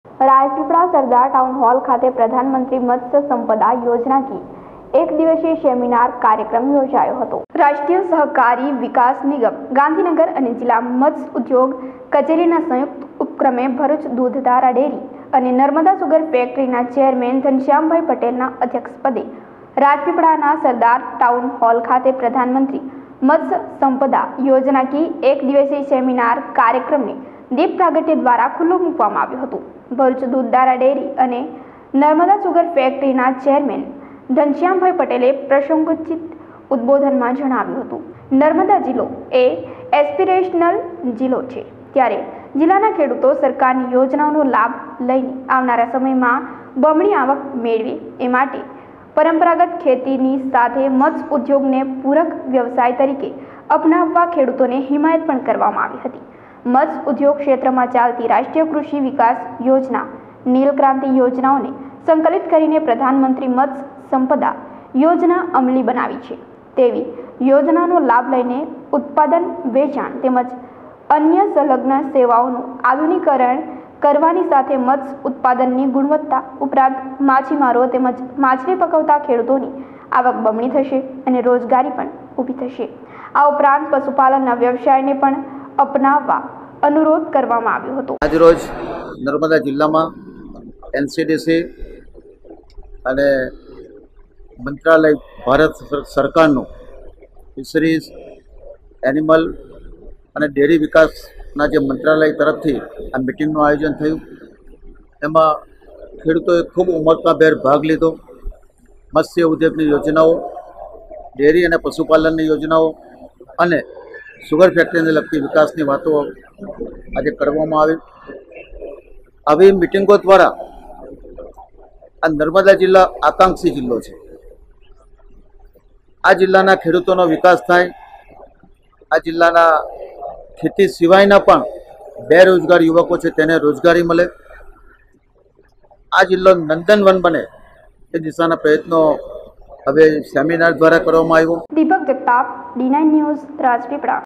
રાજપીપલા સરદાર ટાઉનહોલ ખાતે “પ્રધાનમંત્રી મત્સ્ય સંપદા યોજનાકીય” સેમીનાર યોજાયો
Title:  રાજપીપલા સરદાર ટાઉનહોલ ખાતે “પ્રધાનમંત્રી મત્સ્ય સંપદા યોજનાકીય”  સેમીનાર યોજાયો
Name of the Reporter :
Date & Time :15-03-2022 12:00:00

Descriptions